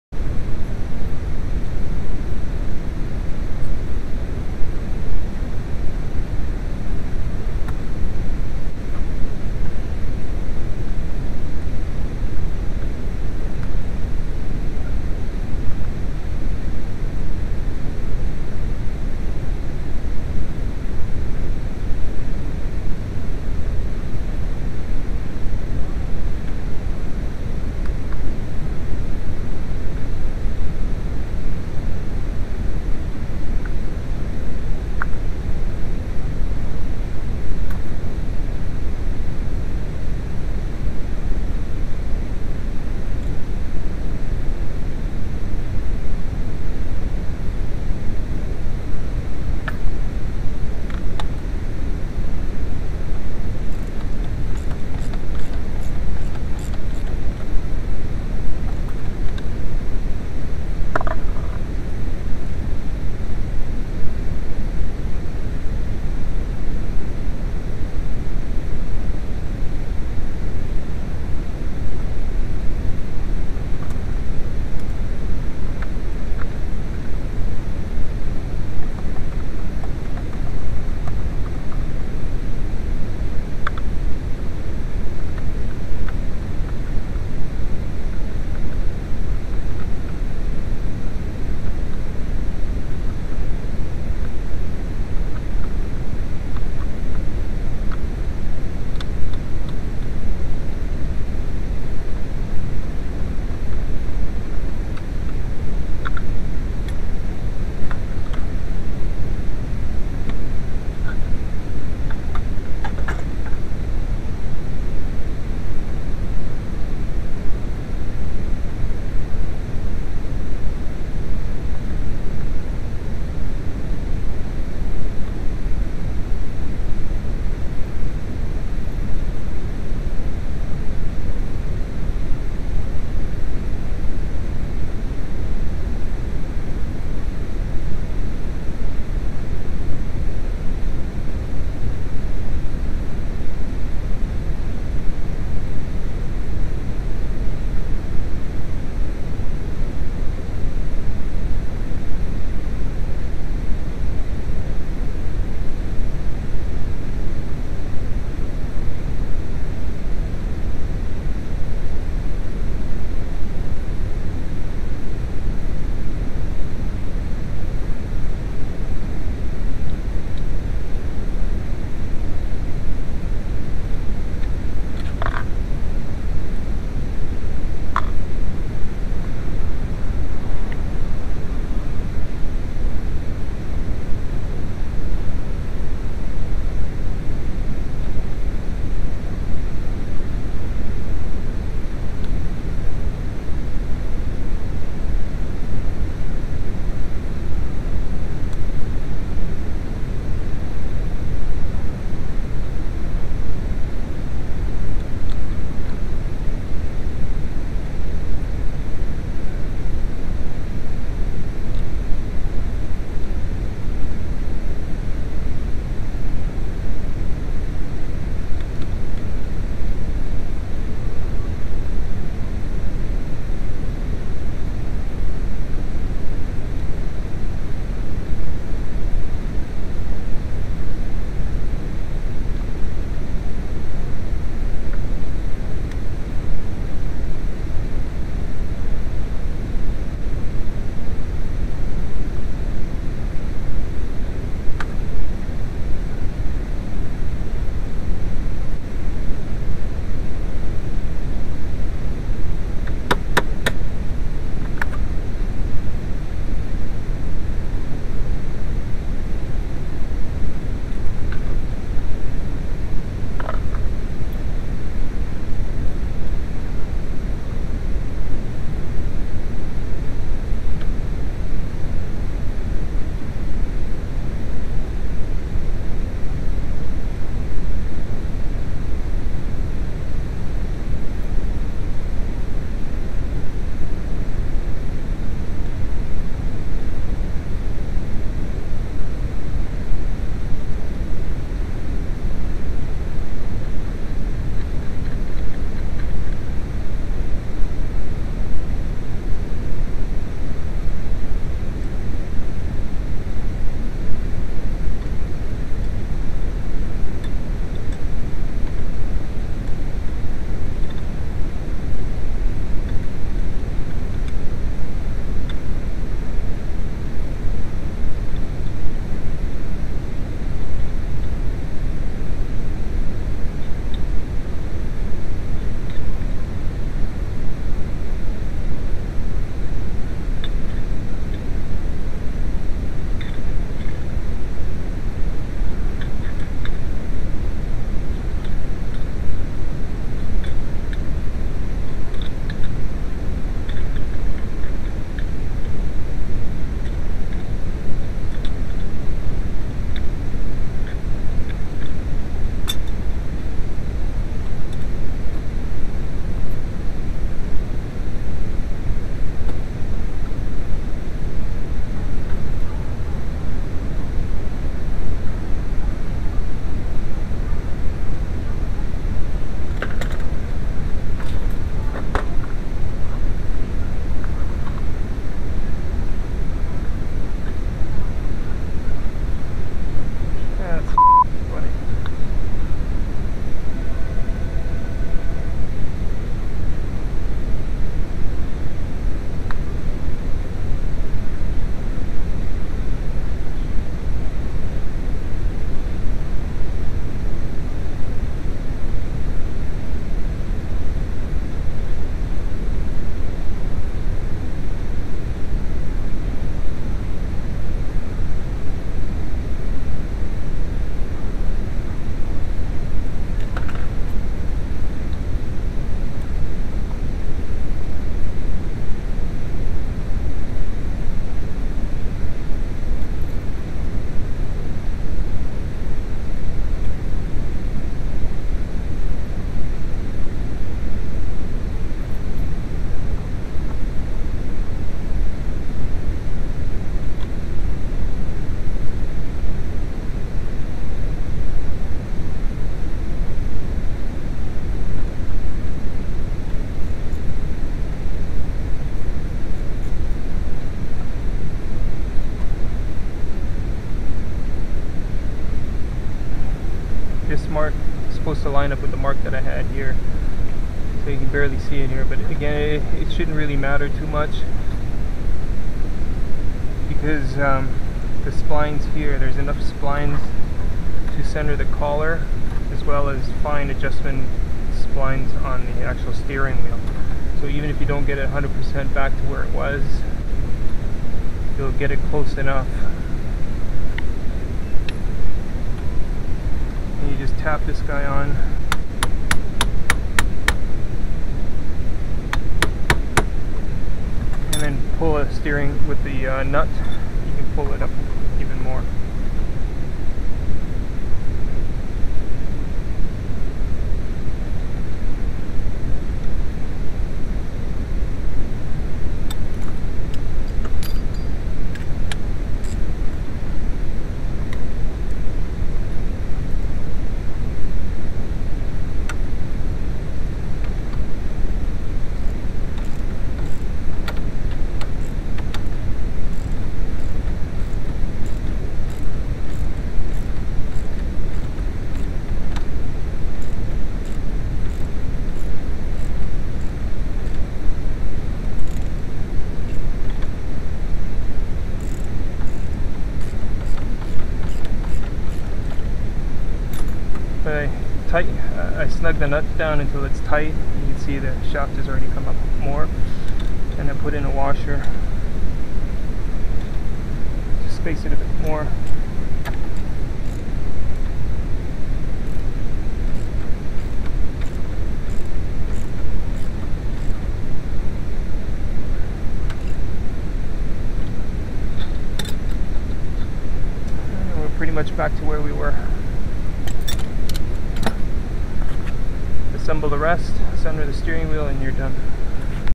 VW A3: Golf / Jetta Ignition Switch Removal & Installation (Standard Transmission)
To change the switch without a puller: (not my DIY Write-up)